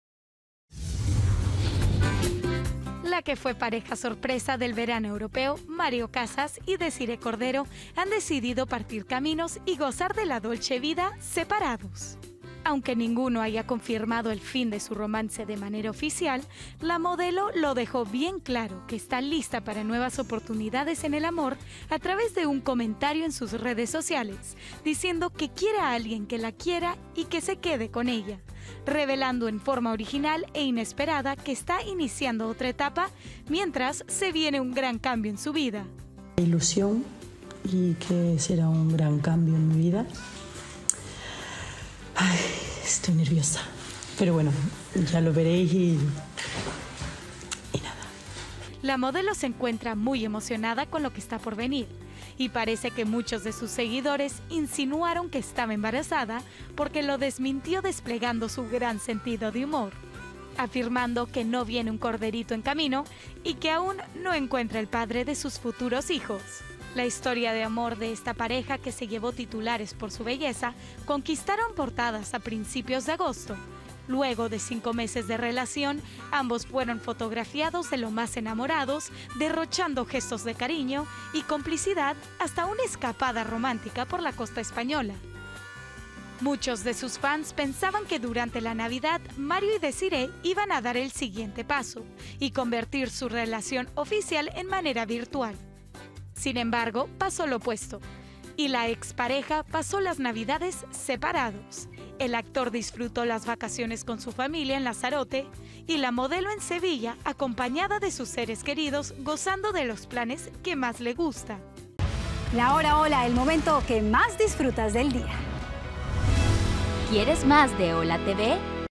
La curiosa forma en la que Desiré Cordero confirmó su ruptura con Mario Casas | ¡HOLA! TV
A través de un video en el que le respondía a un seguidor, se sinceró sobre sus sueños de ser madre, revelando que aún no ha encontrado al hombre ideal para cumplir su meta. Aunque la pareja estuvo viajando por el cantábrico, ambos pasaron las fiestas navideñas por separado en lugares diferentes.